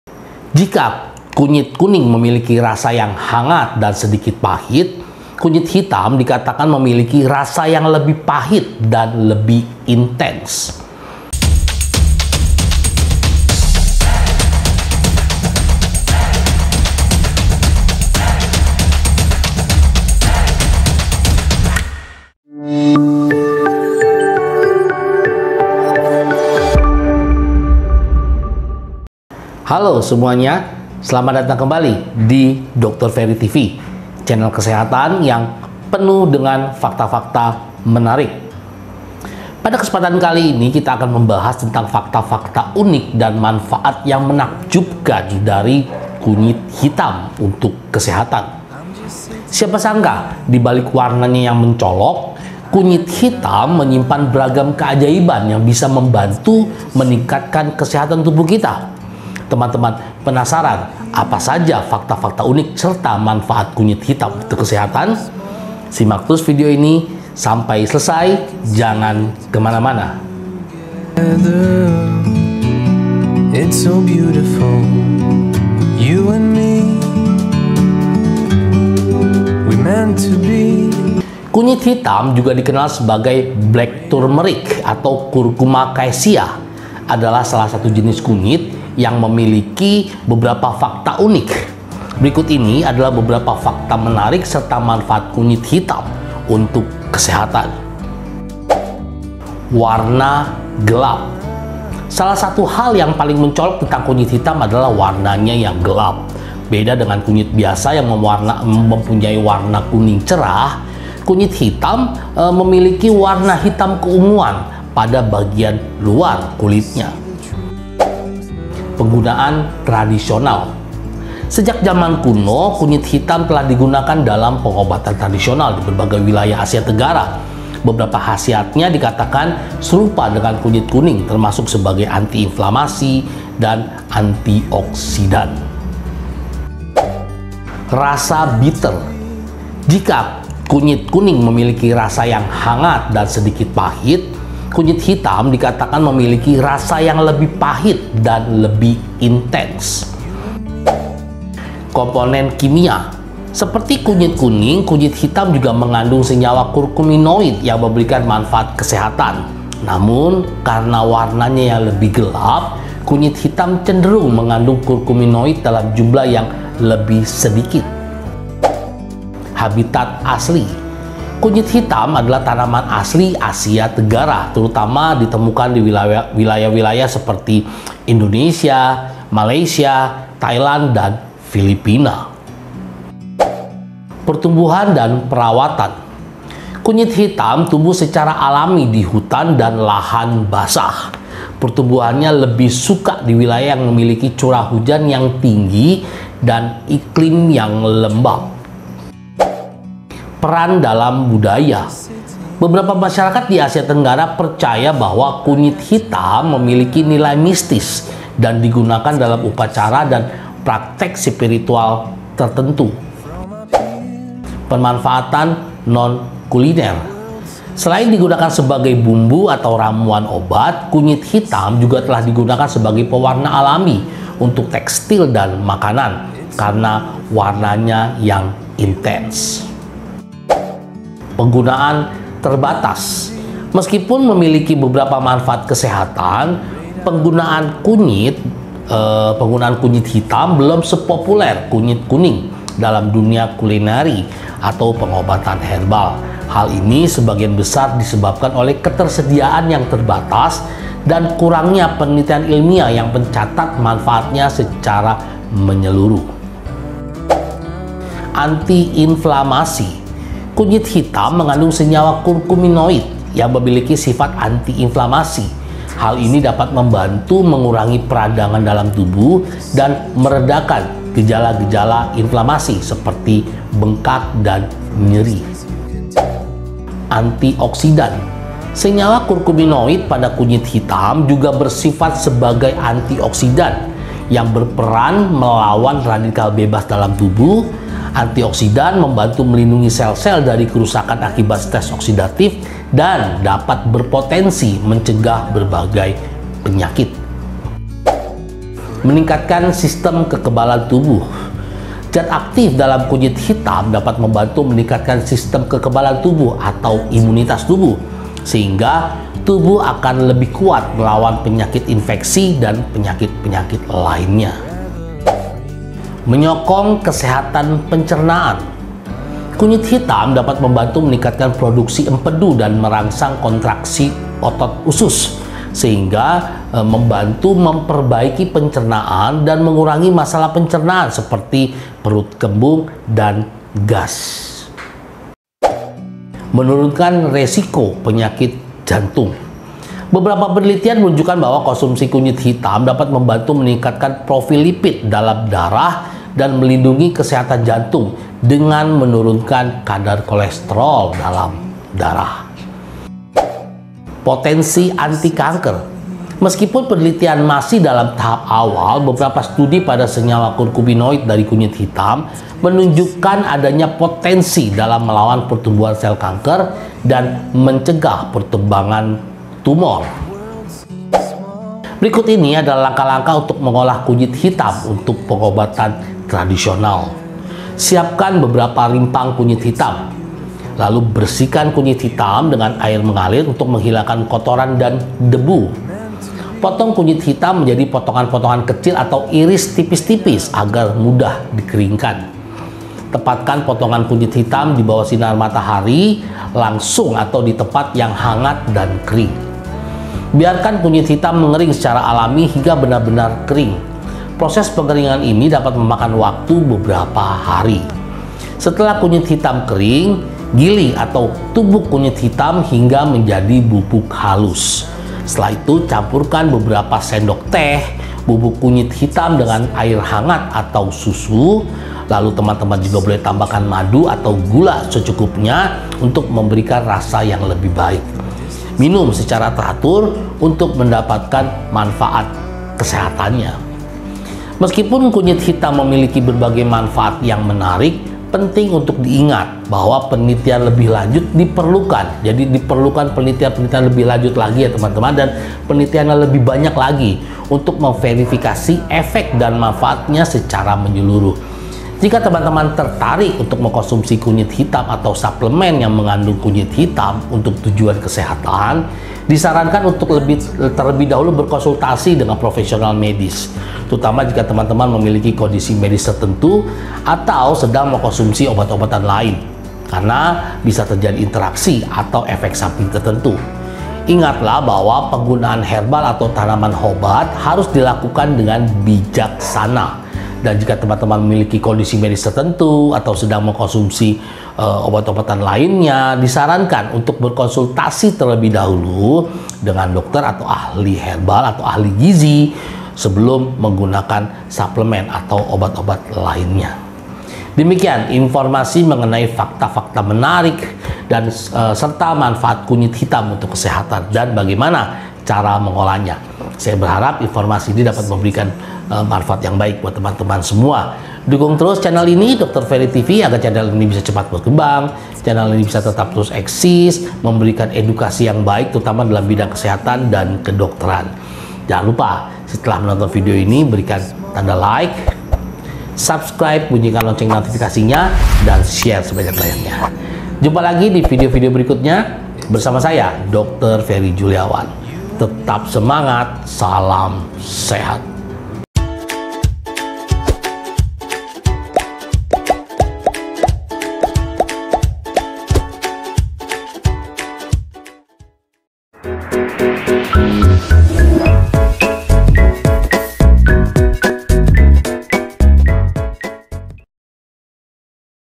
KUNYIT HITAM.............. MANFAATNYA UNTUK KESEHATAN DAN CARA MENGOLAHNYA
FAKTA - FAKTA MENARIK SERTA MANFAAT KUNYIT HITAM UNTUK KESEHATAN

Pada video kali ini kita akan membahas mengenai fakta-fakta menarik serta manfaat kunyit hitam untuk kesehatan dan cara mengolahnya.
simak terus video ini sampai selesai, terima kasih.

salam sehat
dr. Fery Juliawan, M.Th